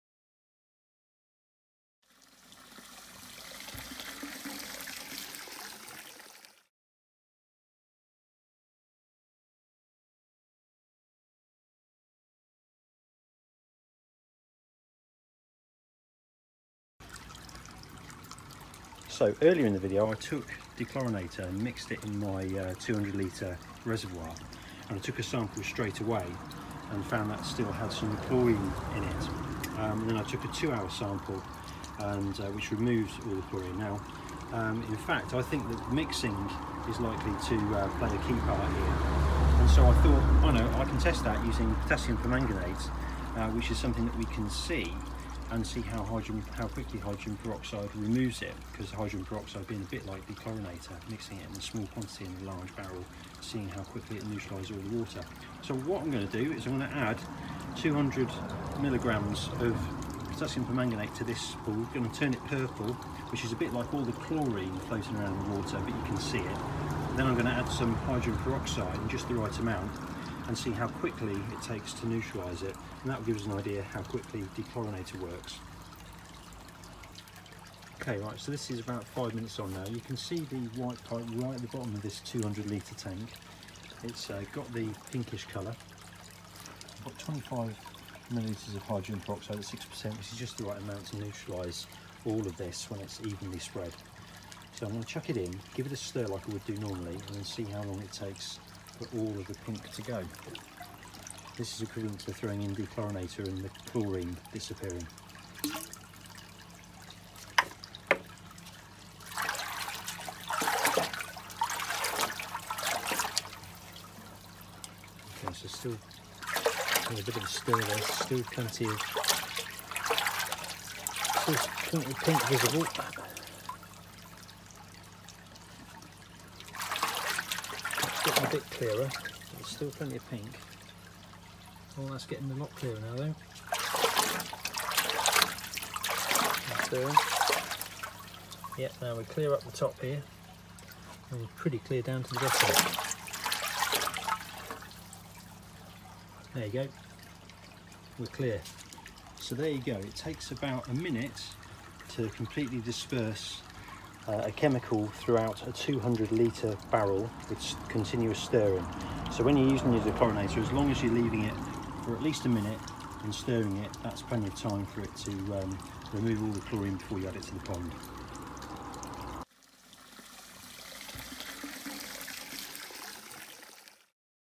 How quickly does dechlorinator work?
Using a purple chemical (PP) that reacts with hydrogen peroxide I show visually how quickly dechlorinator works when you mix it into your tap water